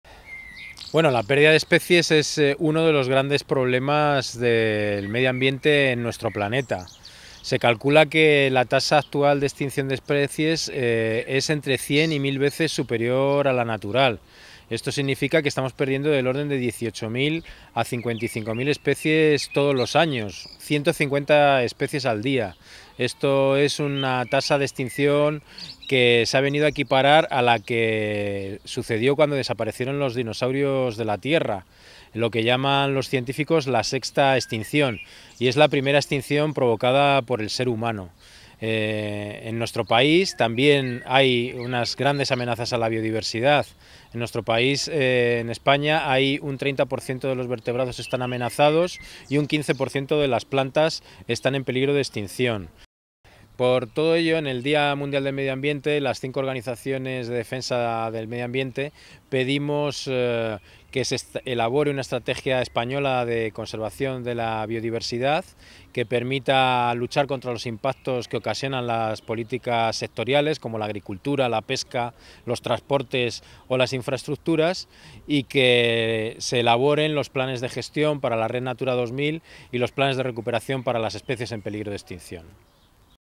Día Mundial del Medio Ambiente, Alejandro Sanchez-Director Ejecutivo SEOBirdLife_web.mov
En el Día del Medio Ambiente, los Directores de las 5 principales ONG ecologistas se unen para lanzar un mensaje frente a la pérdida de biodiversidad